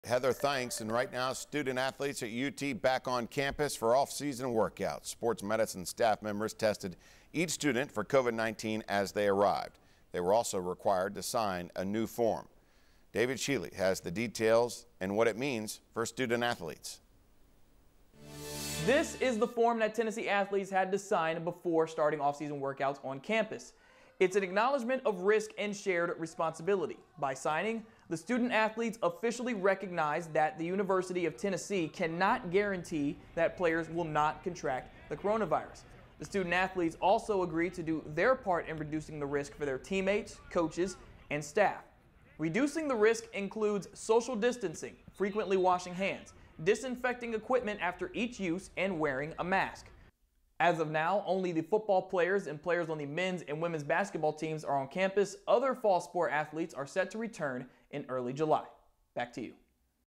UT student-athletes required to sign waiver
Student-athletes at UT are back on campus for offseason workouts.